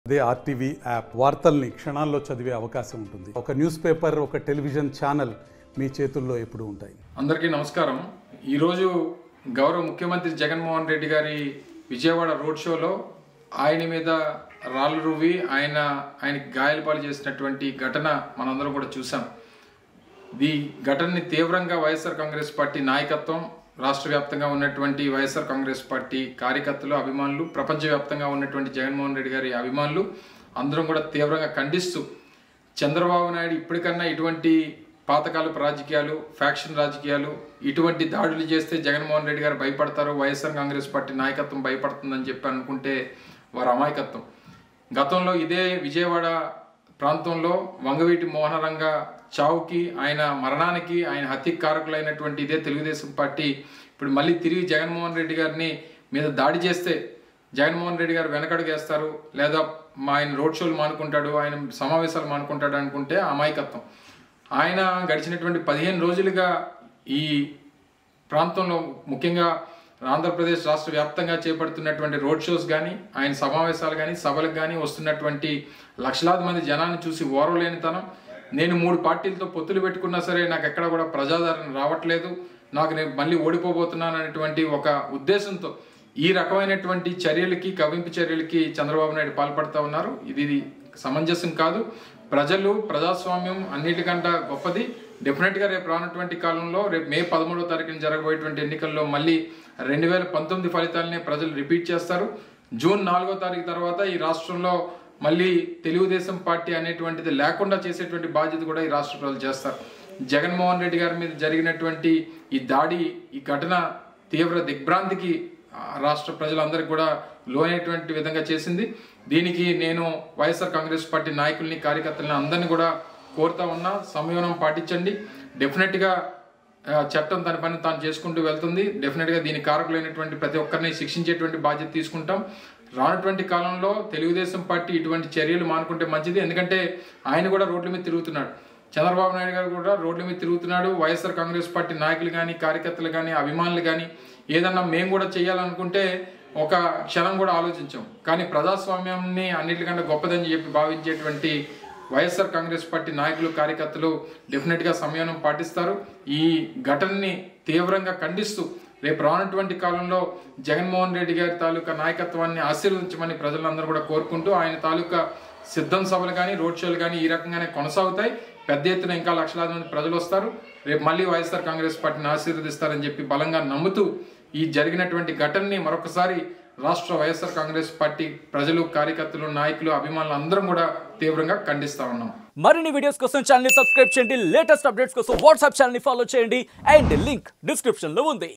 Gudivada Amarnath First Reaction Over Attack On CM Jagan | జగన్ పై దాడి చేసింది వాళ్లే! || RTV
Gudivada Amarnath First Reaction Over Attack On CM Jagan | జగన్ పై దాడి చేసింది వాళ్లే! || RTV Vijayawada